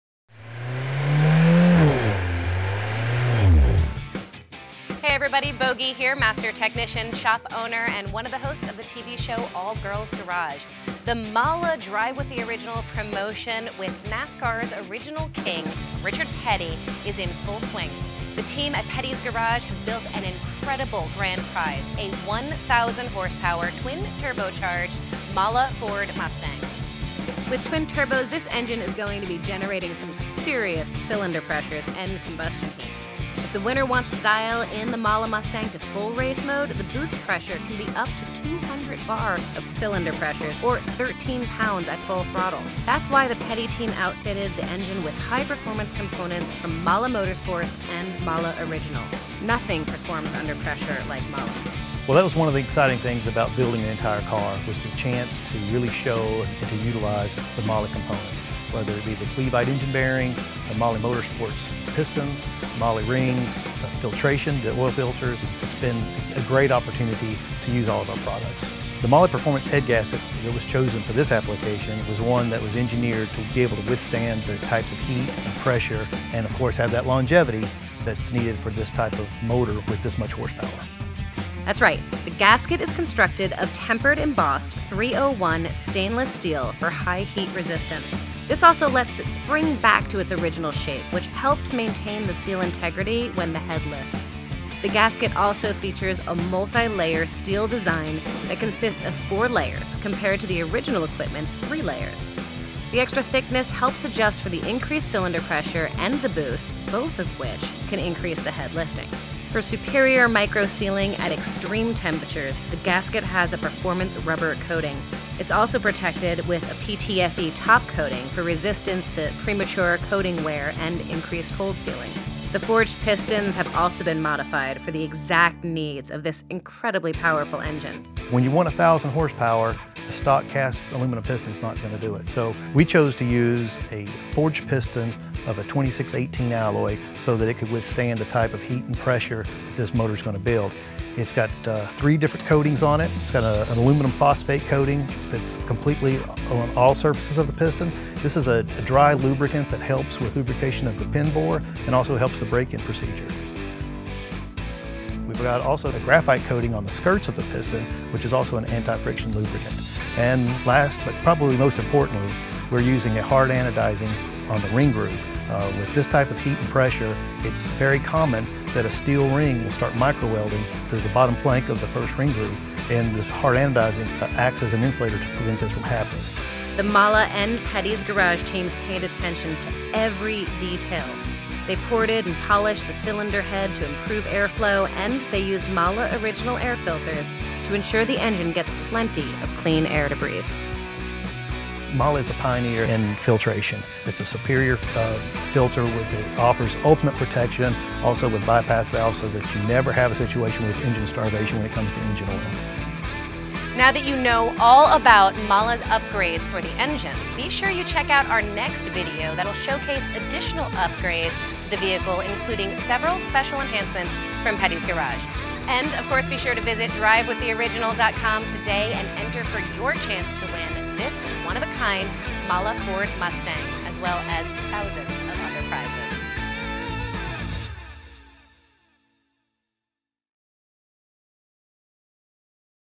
MAHLE Drive With The Original 1000HP Mustang Twin Turbo Engine Build At Petty's Garage
The next chapter in our video series on the 1000HP MAHLE Original Petty's Garage Drive With The Original Ford Mustang giveaway car, shows us how Petty's team built this beast of an engine using our superior technology MAHLE Original Performance Gaskets, Clevite Bearings, MAHLE Motorsport North America Pistons, Rings, and MAHLE Filters. To sign up to win this car or one of thousands of instant win prizes, and purchase MAHLE Original Gaskets for more entries!